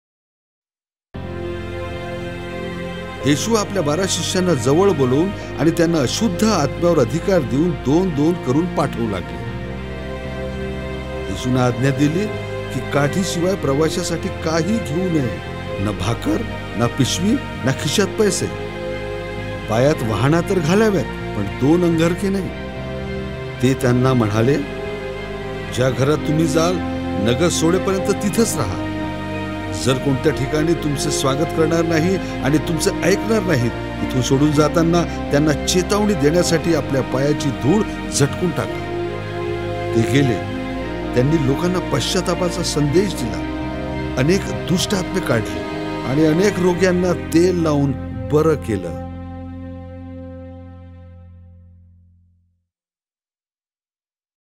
051 - Twelve Disciples Sent Out (Marathi)
Scripture Reference: Mark 6: 7-13
Jesus sends His disciples out and gives them the power over unclean spirits and to heal.

Question 1: What can we learn from the instructions Jesus gave the twelve disciples?

Question 2: What could you, or would you, be willing to do to help share the Message of Christ?

Question 3: By what methods can the Church effectively preach the need for repentance to those where you live?